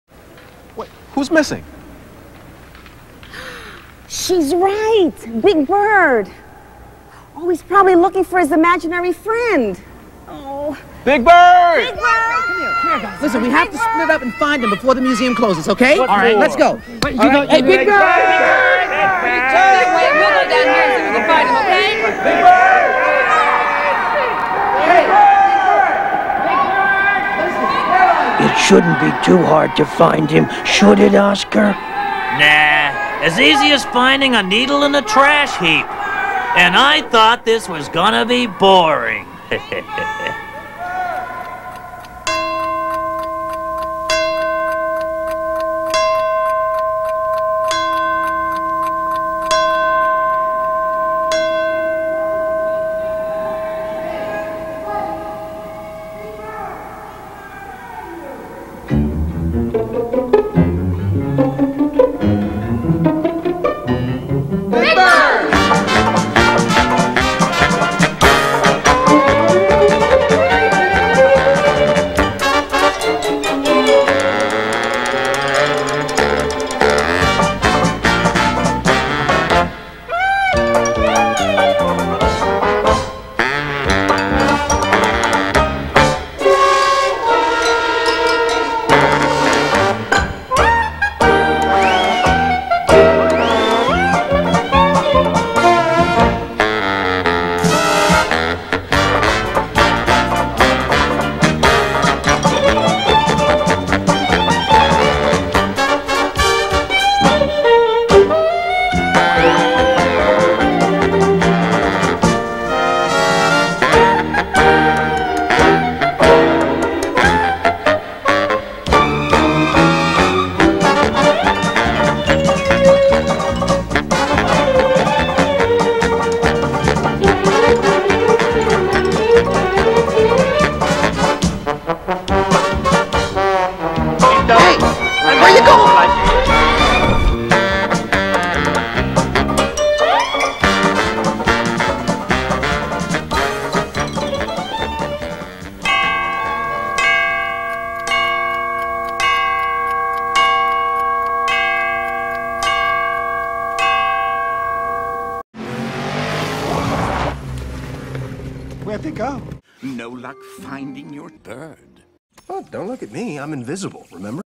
Chasing Big Bird Version 3 (a Ren the God of Humor crossover)
They say three's a charm. So here's Version 3 of the very first video I made with clips. All your favorite characters new and old are running around the Metropolitan Museum of Art searching for Big Bird. Think they'd take a rest by now.